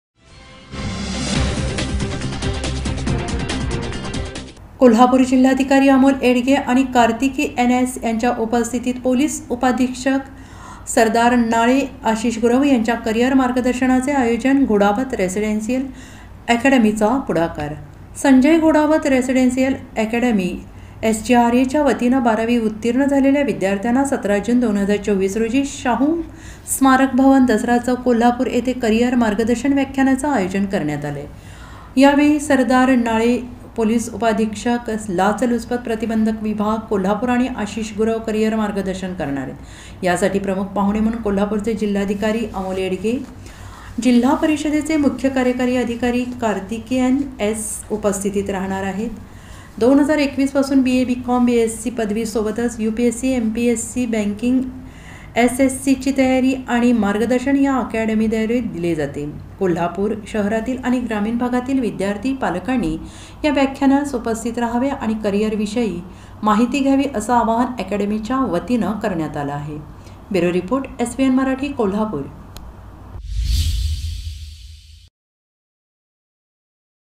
KOLHAPUR | जिल्हाधिकारी अमोल येडगे व कार्तिकीएन एस.यांच्या उपस्थित करिअर मार्गदर्शनाचे आयोजन
कोल्हापूर जिल्हाधिकारी अमोल येडगे व कार्तिकीएन एस. यांच्या उपस्थित पोलीस उपाधीक्षक सरदार नाळे, आशिष गुरव यांच्या करिअर मार्गदर्शनाचे आयोजन, घोडावत रेसिडेन्सील अकॅडमीचा पुढाकार